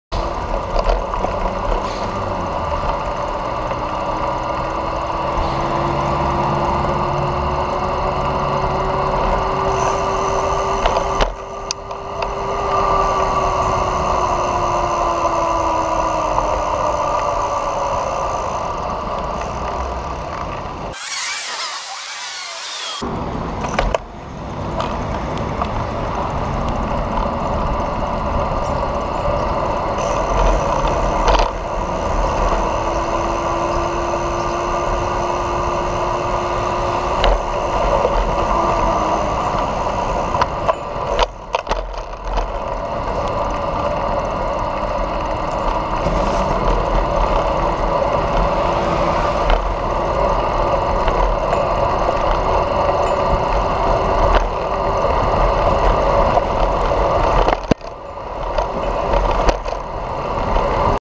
Joan Güell un dia de vaga bicing
carrilbici Joan Güell, dia laborable amb vaga bicing la gent va en bicibcn tranquil·lament + bonus conductor respectuós